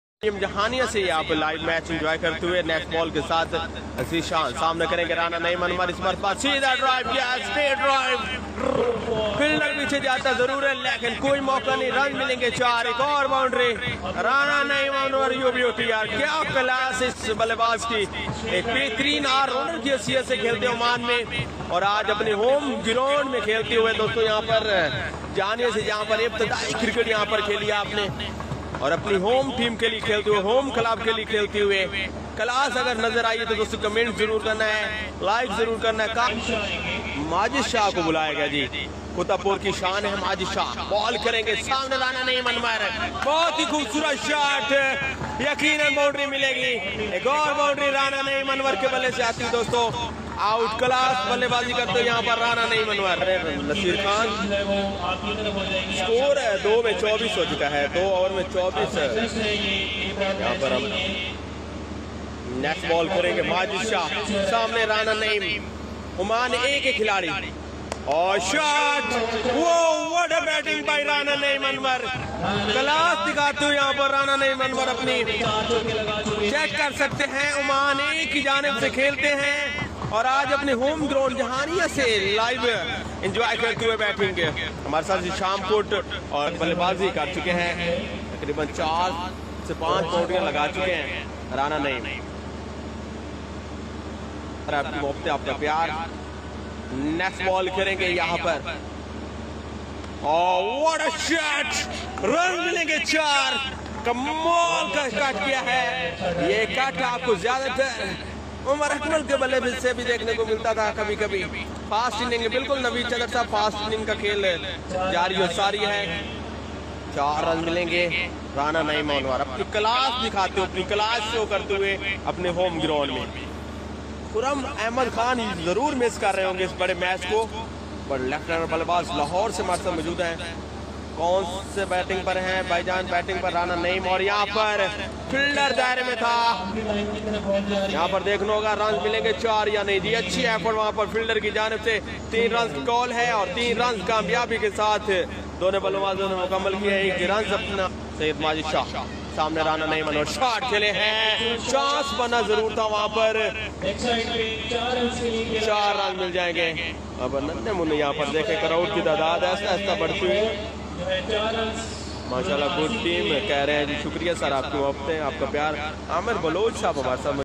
Rana Naeem Playing in home Ground
1st Floodlight T20 Match in the History of Jahanian khanewal and Lodhran
Jahanian Tiger's Cricket club Registered Vs Shaheen Cricket club Qutabpur